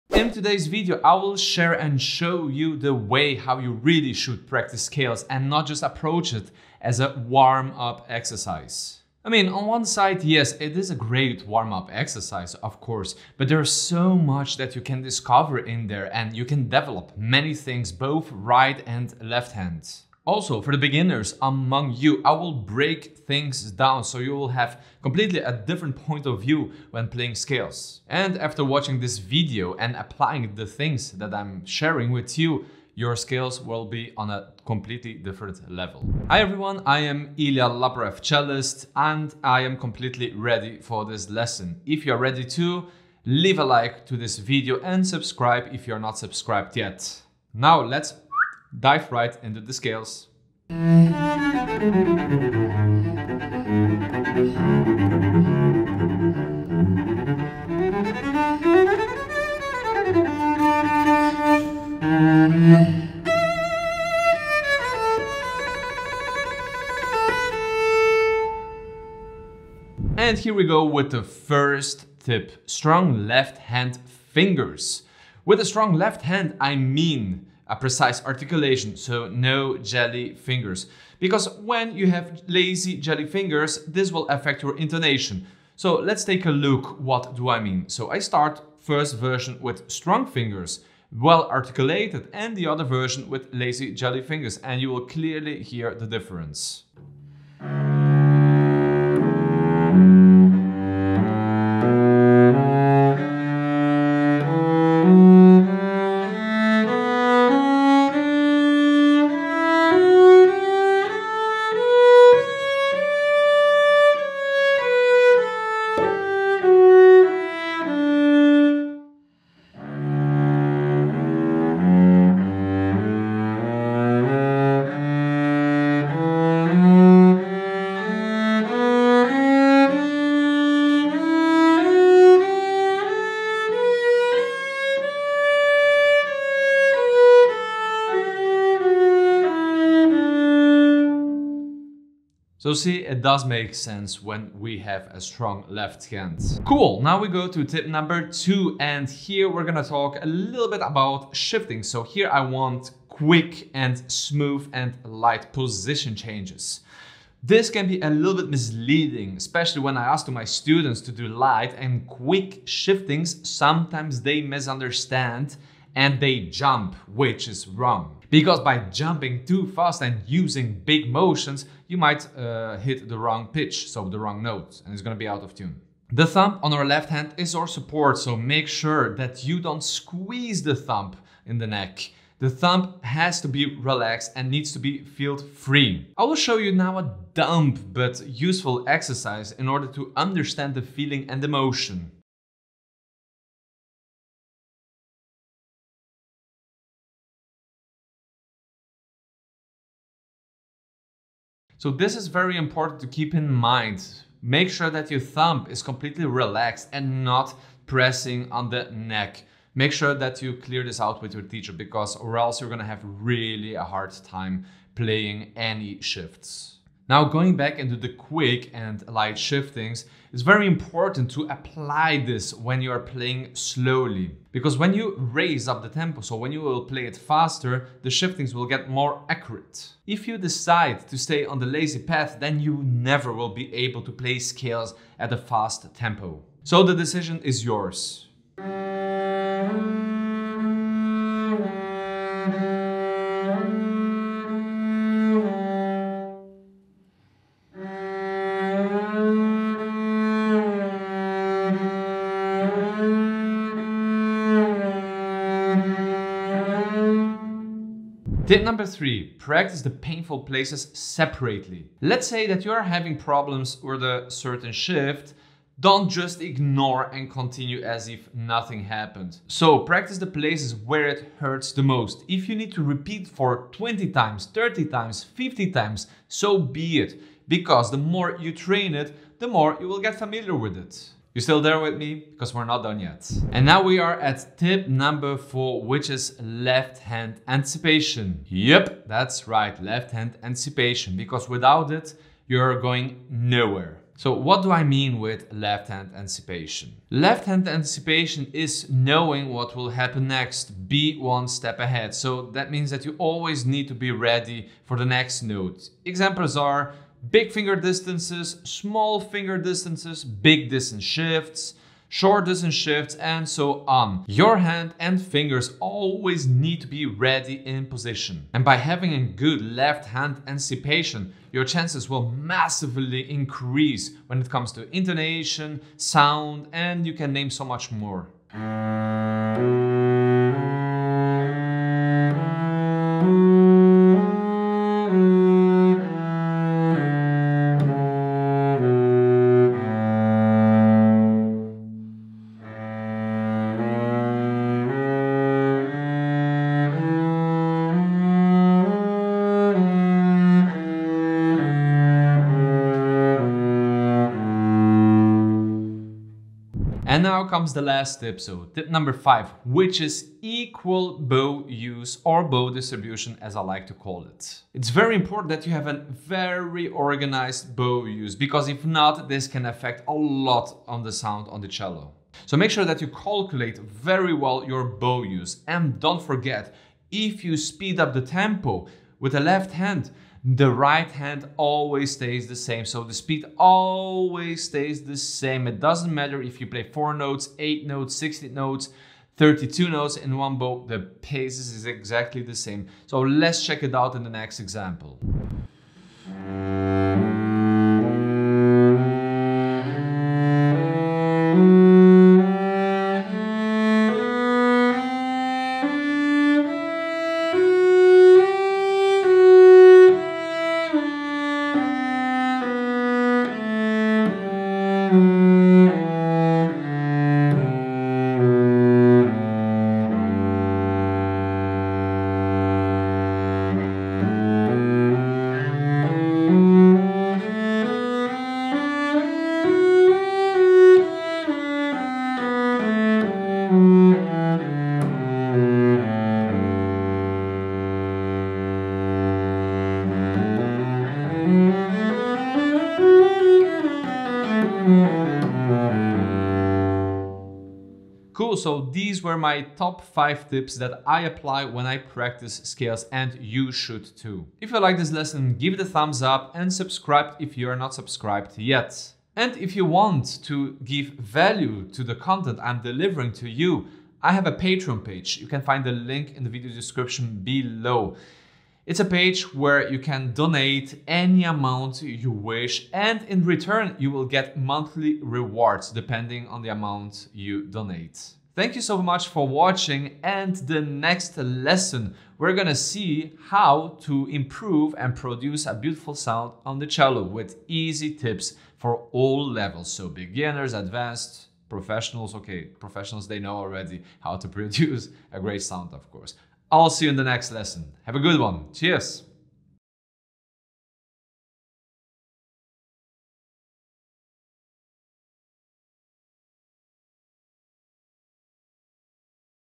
Cello Scales - Problems & FIX Them in 8 Minutes!
Today we check out the 5 worst technique mistakes and problems in Cello Scales and how to fix them!

With these cello tips, they will help you with fixing bad technique habits in your practice routine! It will greatly help you with finally playing the music that you love! What's your biggest bad habit or frustration? Make sure to share it in the comments!

🎻 1 to 1 PREMIUM Cello Masterclasses with Ilia Laporev!

0:00 Introduction
0:50 Tip #1
2:26 Tip #2
4:18 Tip #3
4:42 Tip #4
5:57 Tip #5
7:28 Outro